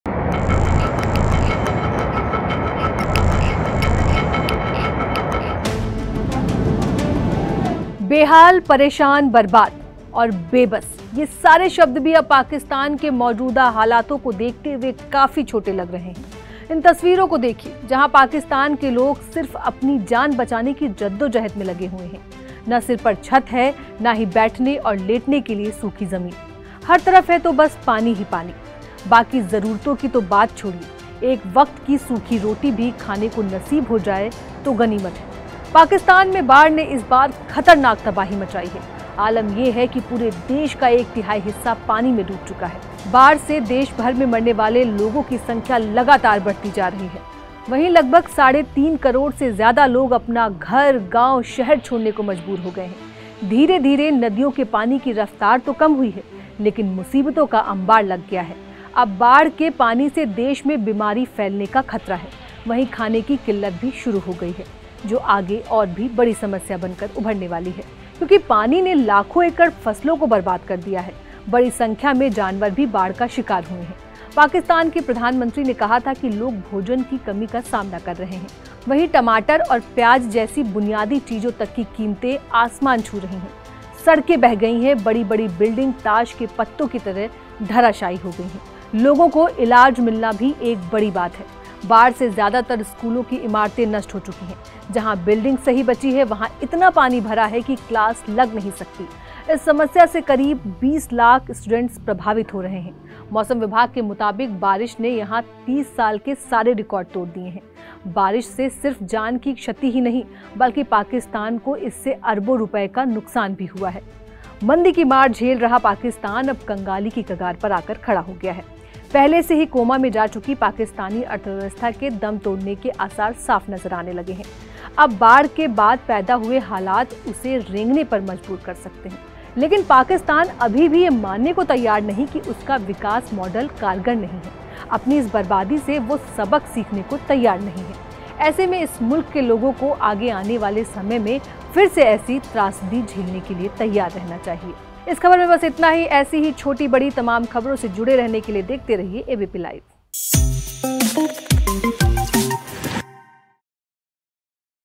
बाढ़ ने Pakistan को बनाया कंगाल, जानिए कैसे हो गए हैं वहां के हालात ?
पाकिस्तान के लोग सिर्फ अपनी जान बचाने की जद्दोजहद में लगे हुए हैं. न सिर पर छत है. न ही बैठेने और लेटने के लिए सूखी जमीन हर तरफ है तो बस पानी ही पानी, बाकि जरूरतों की तो बात छोड़िए एक वक्त की सूखी रोटी भी खाने को नसीब हो जाए तो गनीमत है. पाकिस्तान में बाढ़ ने इस बार खतरनाक तबाही मचाई है. आलम ये है कि पूरे देश का एक तिहाई हिस्सा पानी में डूब चुका है. बाढ़ से देश भर में मरने वाले लोगों की संख्या लगातार बढ़ती जा रही है. वहीं लगभग साढ़े तीन करोड़ से ज्यादा लोग
अपना घर, गांव, शहर छोड़ने को मजबूर हो गए हैं.